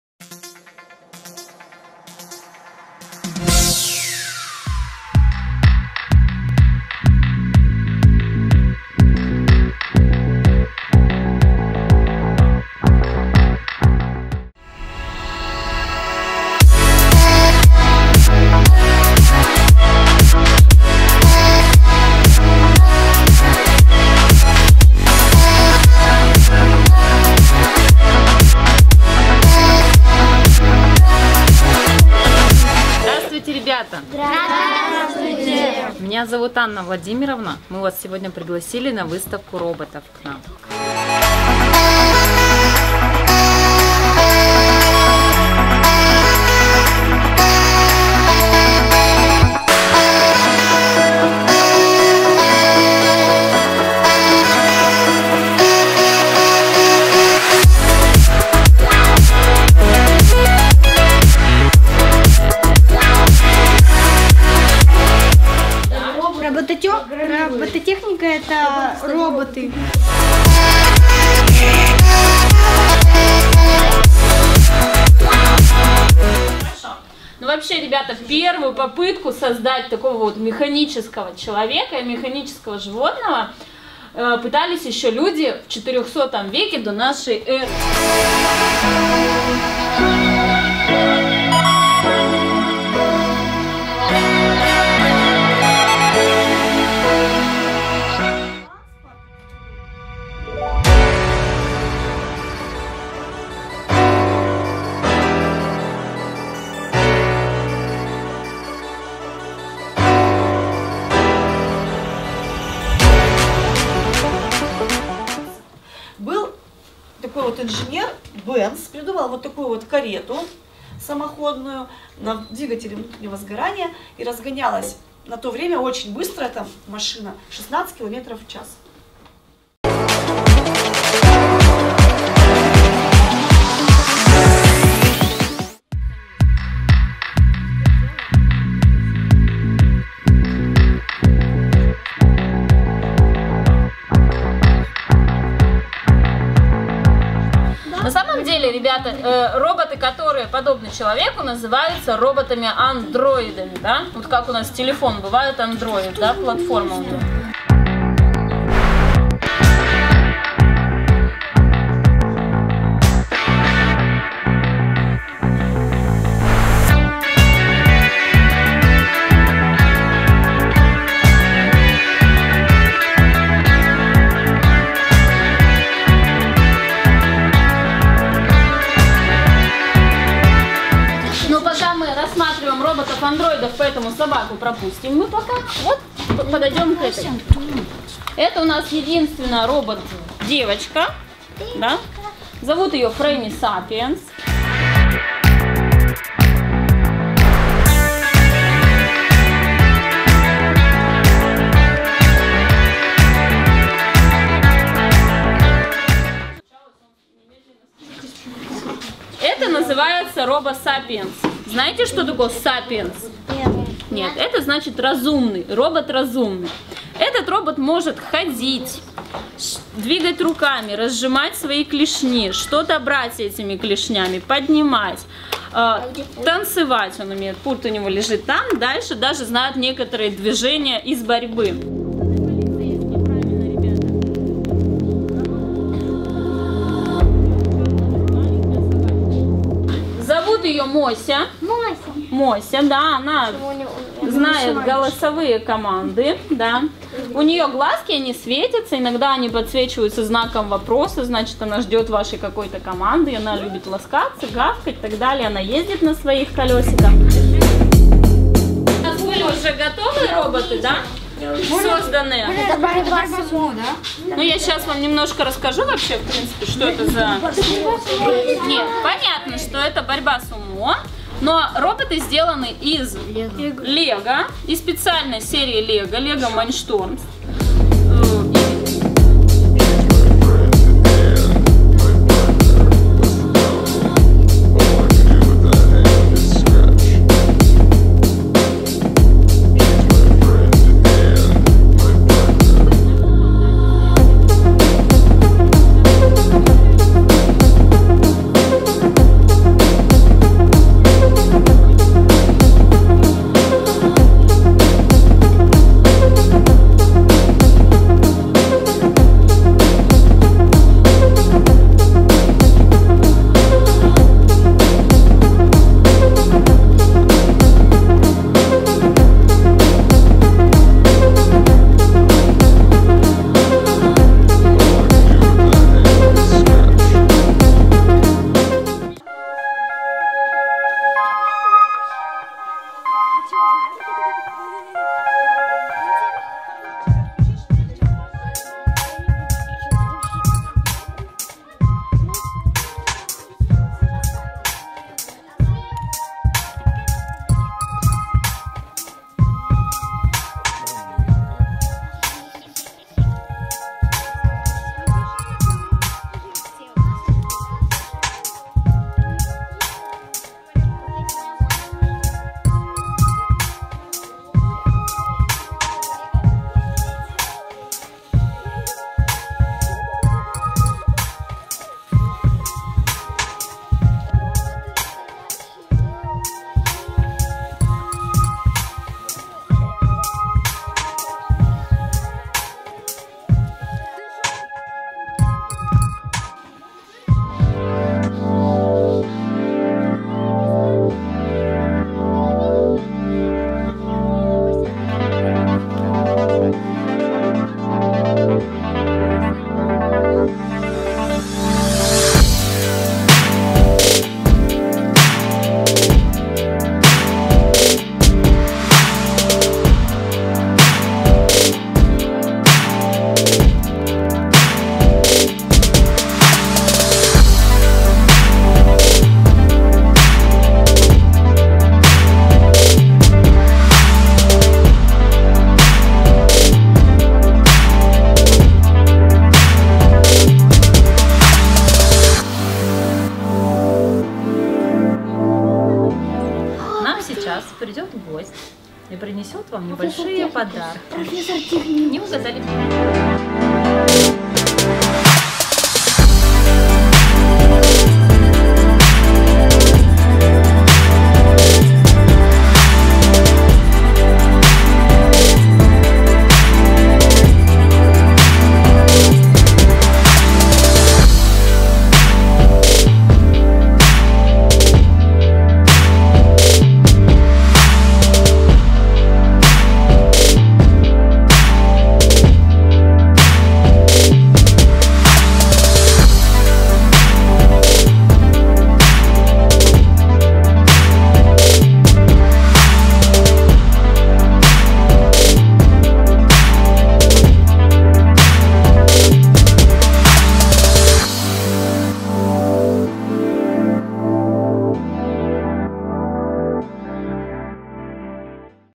СОЗДАНИЕ РОБОТОВ! РОБОТЕКС. ИСТОРИЯ.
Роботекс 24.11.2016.
Учение и развлечение для ребят 3-го класса. Занимательная физика и механика.